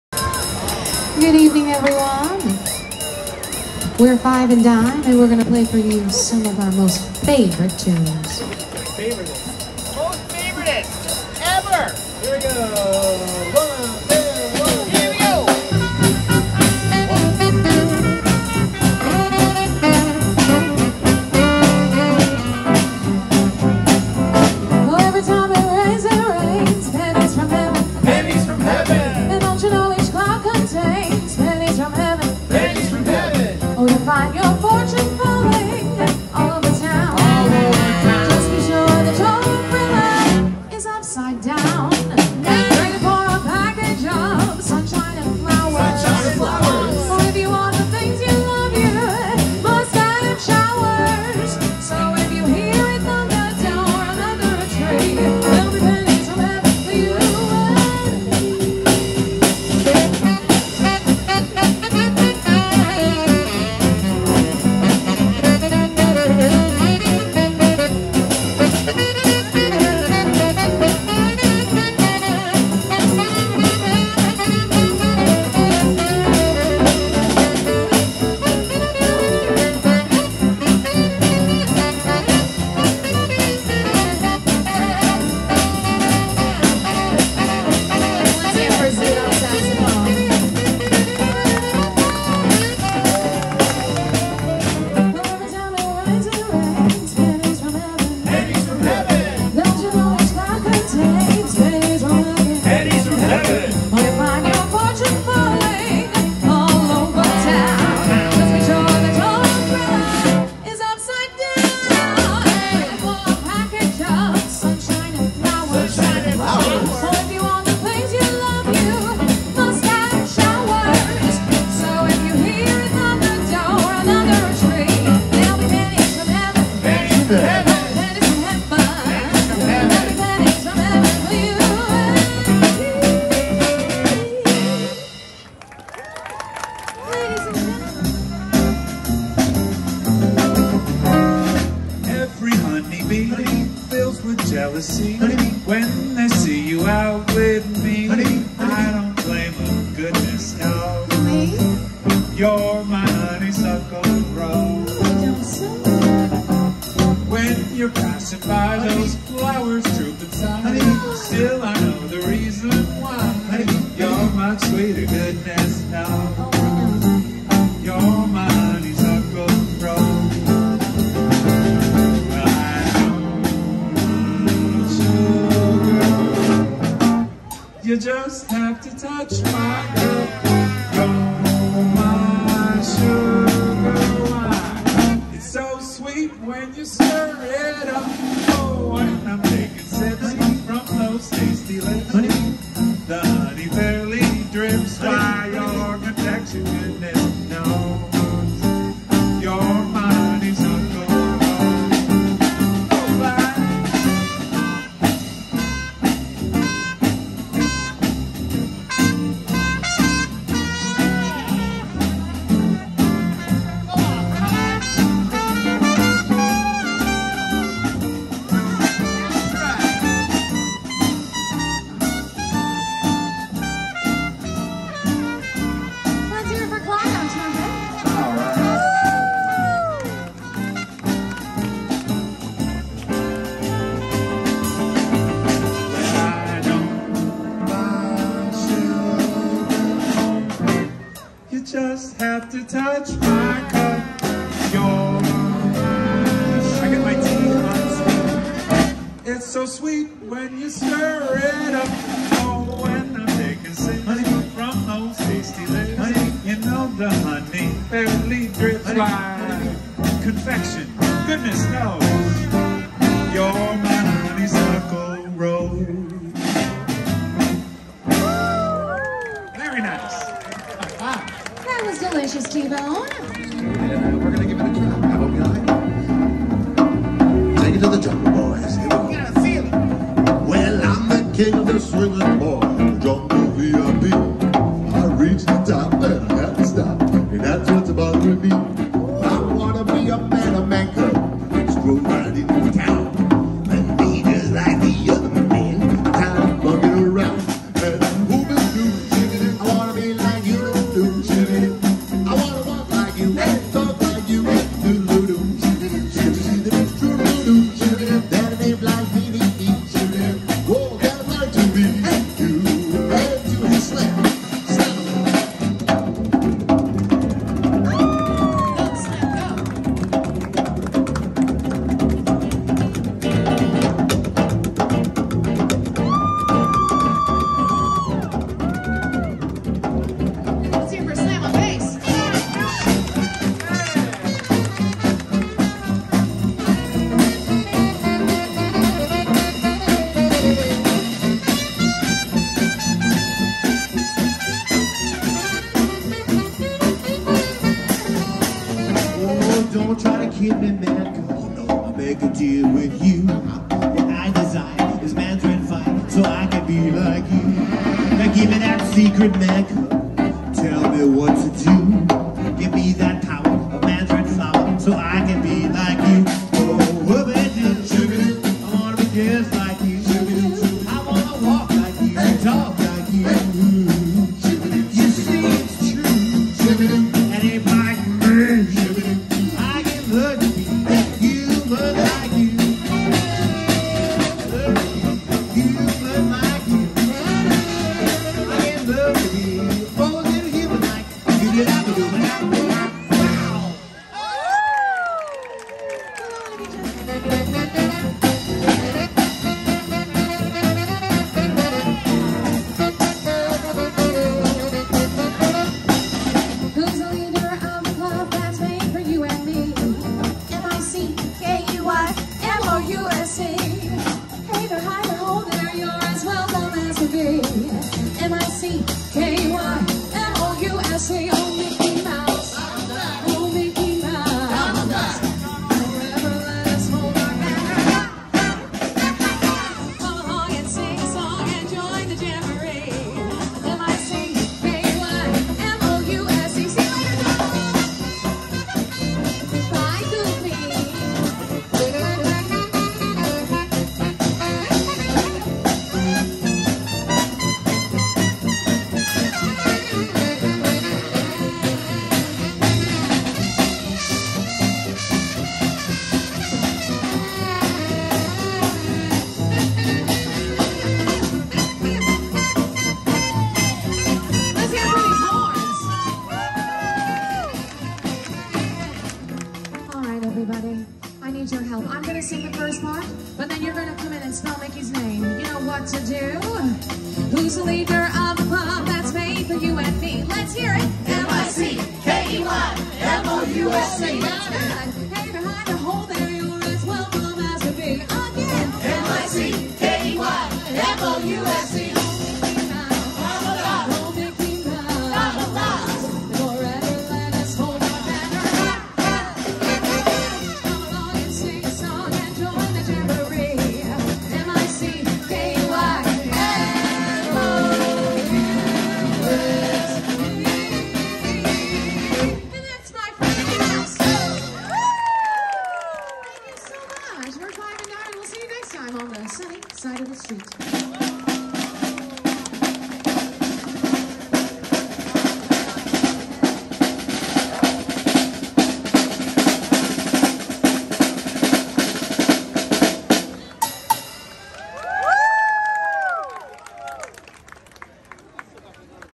Disney’s California Adventure: Five and Dime Live Band - Nighttime Set
Jazz music: Live music entertainment at Disney’s California Adventure.